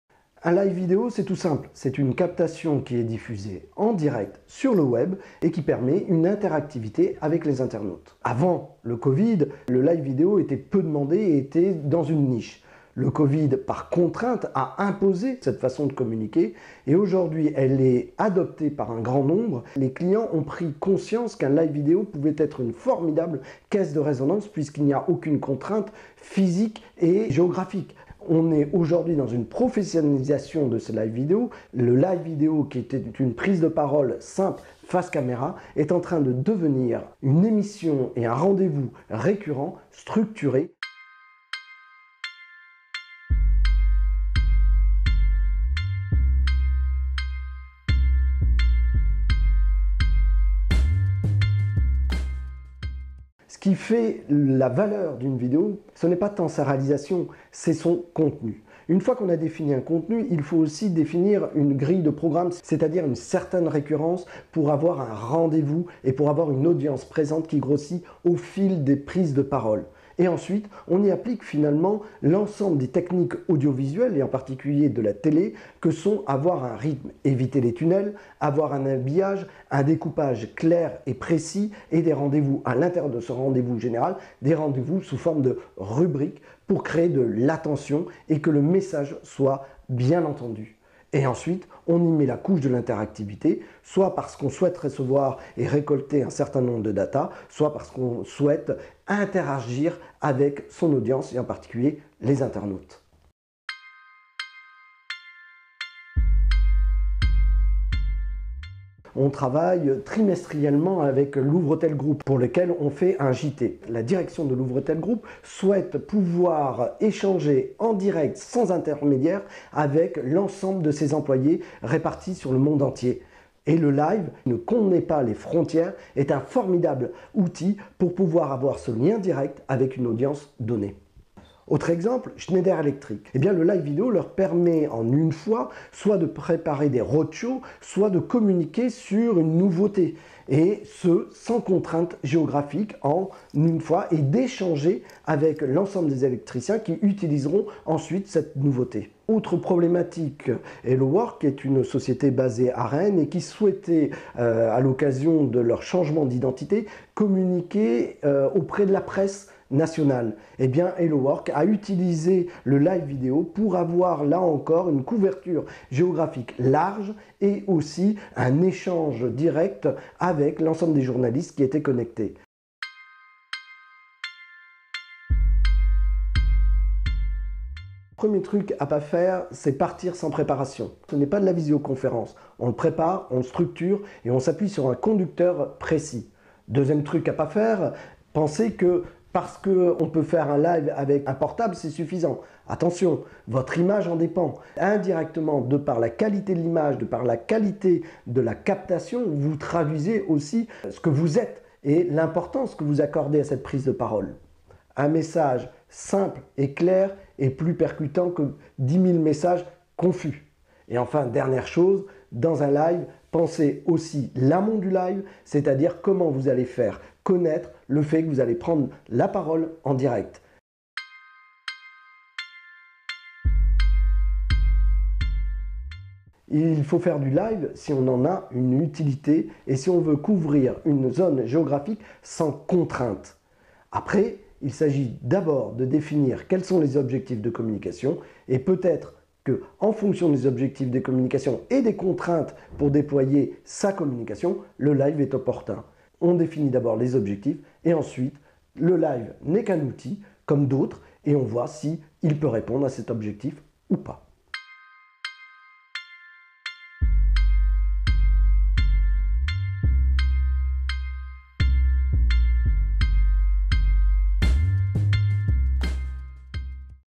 [Fr] La professionnalisation du Live Vidéo avec François Sebastia — Kera Groupe
Un live vidéo, c'est tout simple, c'est une captation qui est diffusée en direct sur le web et qui permet une interactivité avec les internautes. Avant le Covid, le live vidéo était peu demandé et était dans une niche. Le Covid, par contrainte, a imposé cette façon de communiquer et aujourd'hui, elle est adoptée par un grand nombre. Les clients ont pris conscience qu'un live vidéo pouvait être une formidable caisse de résonance puisqu'il n'y a aucune contrainte physique ni géographique. On est aujourd'hui dans une professionnalisation de ce live vidéo. Le live vidéo, qui était une prise de parole simple face caméra, est en train de devenir une émission et un rendez vous récurrent et structuré.